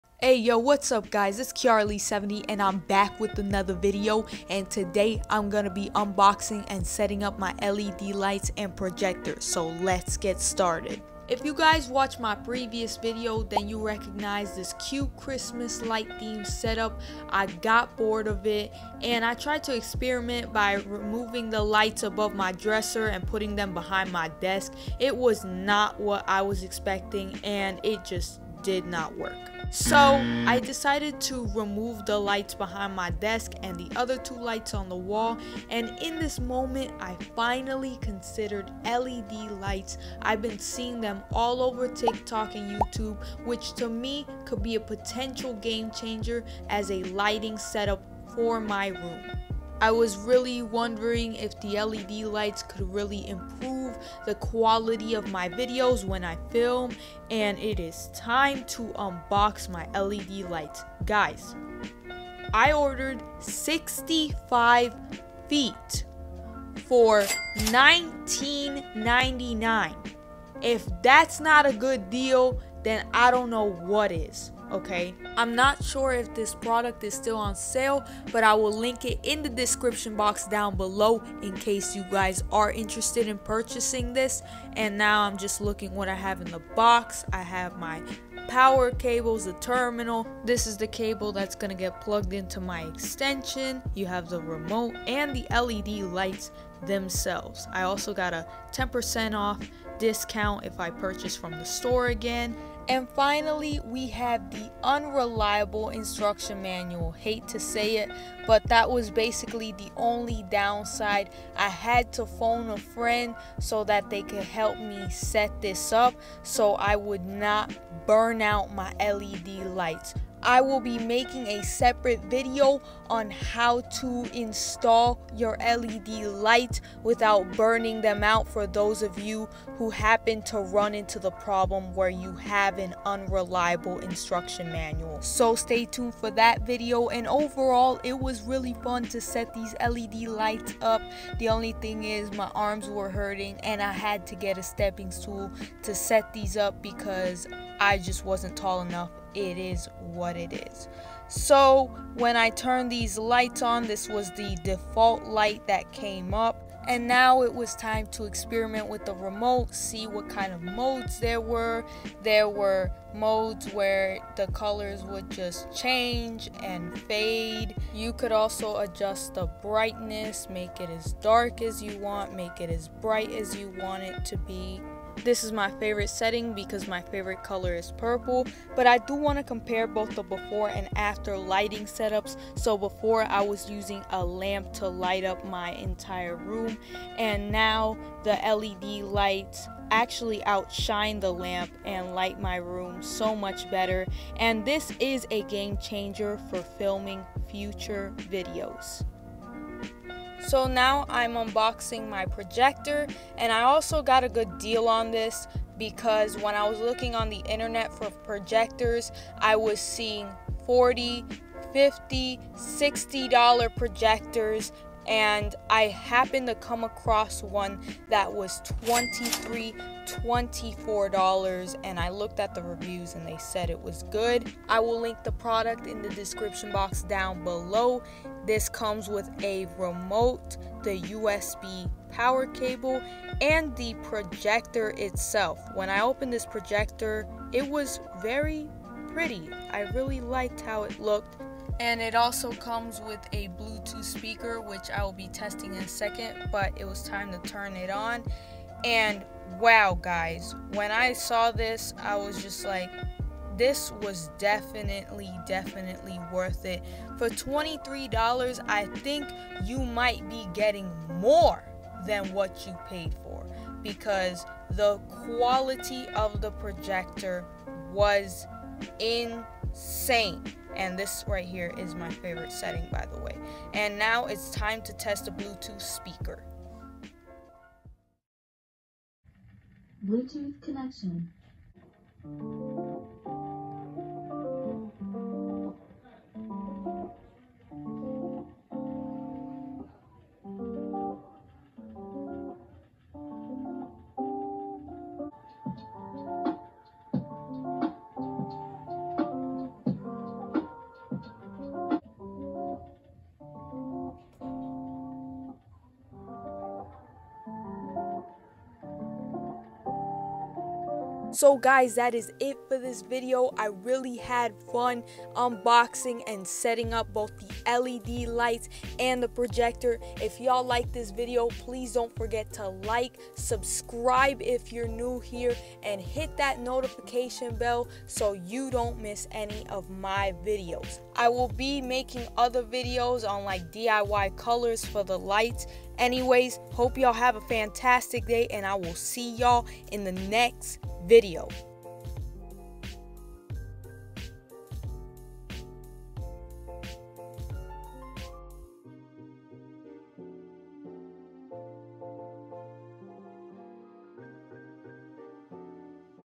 Unboxing and Installing Dalattin LED Strip Lights 65.6 ft and Starlight Projector!! (2021 Review)
Hey guys in this video, I will be installing affordable LED lights along with a Starlight projector from Amazon! Hope you enjoyed the video!!!

Dalattin LED strip Lights:

Star Light Projector

STRAIGHTENING MY CURLY HAIR + REVIEW of Remington Pro Wet 2 Style Dryer

How to Setup Dalattin LED Strip lights When your Instruction Manual SUCKS!